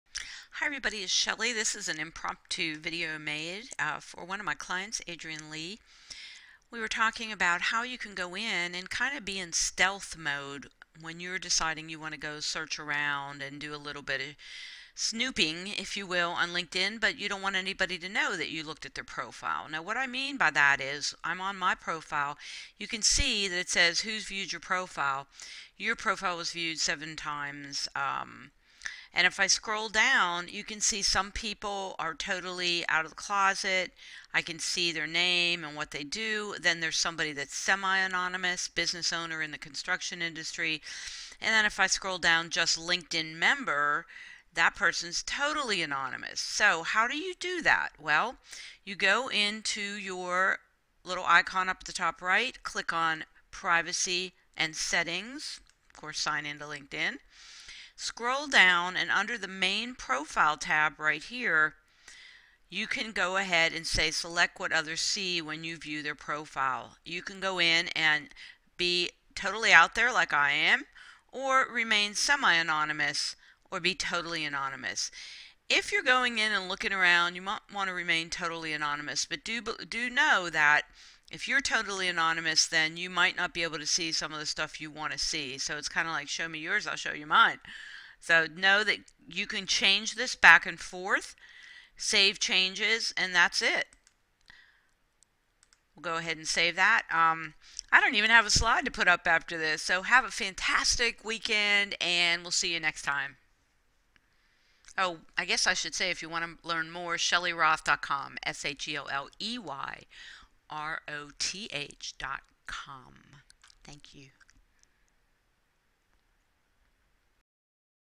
Be in Stealth Mode on LinkedIn
Want ot search around LinkedIn and stay anoyomous? Watch to learn how.